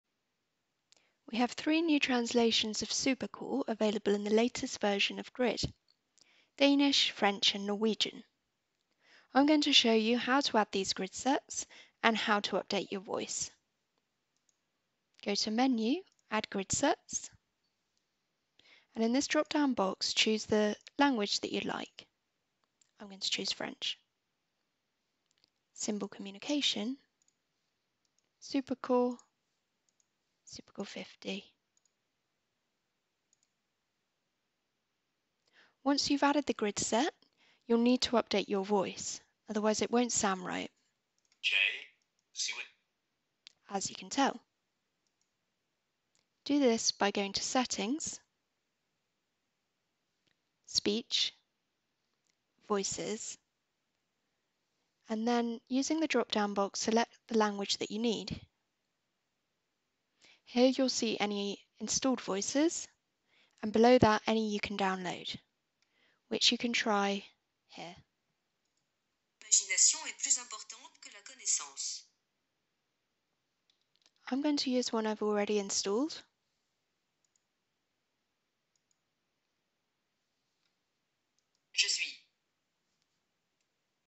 How to add a translated version of Super Core
The latest Grid update now includes Danish, French and Norwegian translations of Super Core.

To add a translated version of Super Core, go to Add grid sets, select the relevant language from the drop down menu and choose Symbol communication - Super Core.